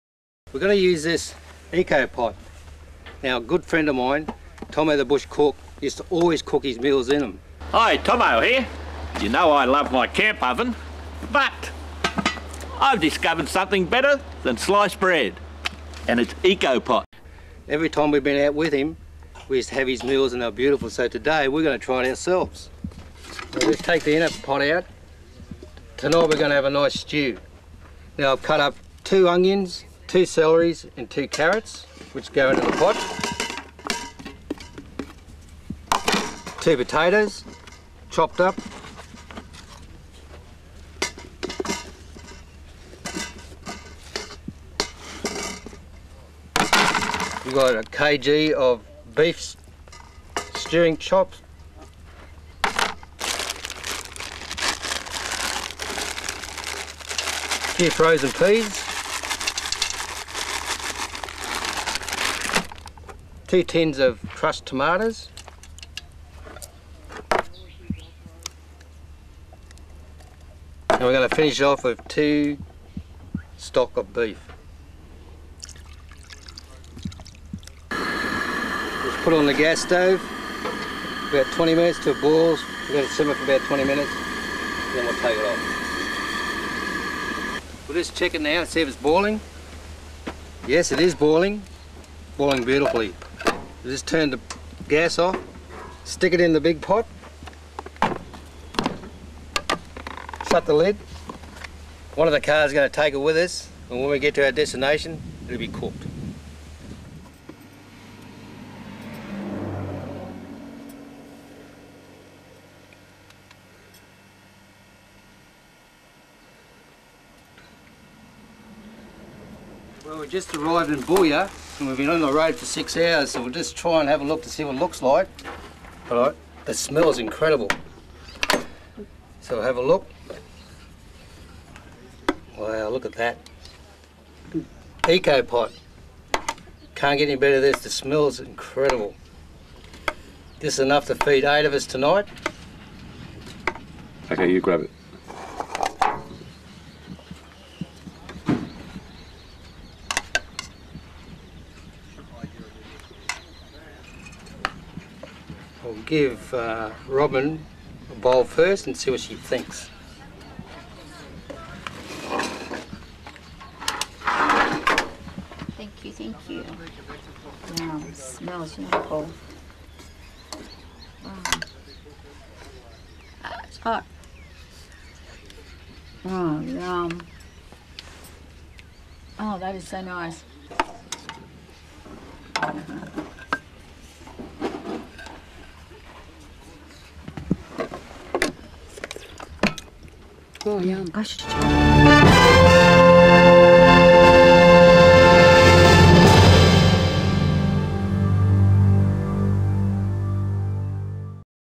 ECO POT OLD CORK
Prepare food in ecopot in the morning then traveled all day had a hot meal that night very good